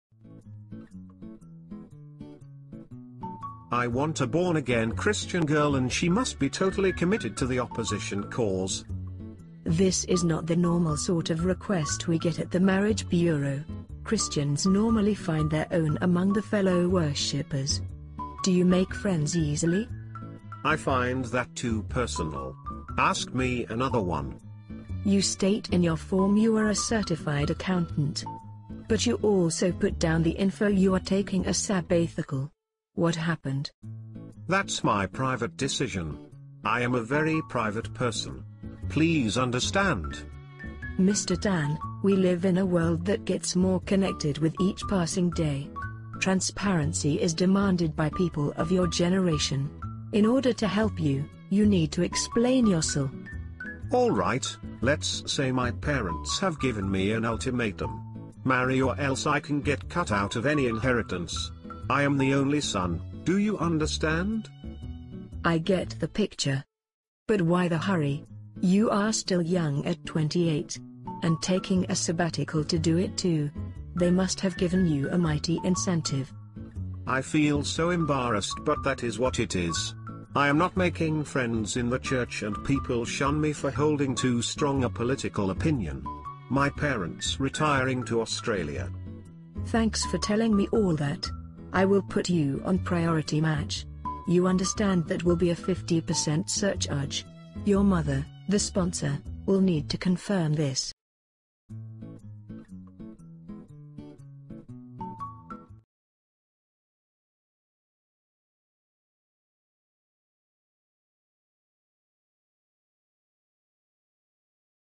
Only son must marry or else
Parents consciously or unconsciously want to live through the lives of their children and passing on the family name can become an obsession. When things do not happen as wished, parents can make their wishes known and may offer a carrot as well as stick to enforce them!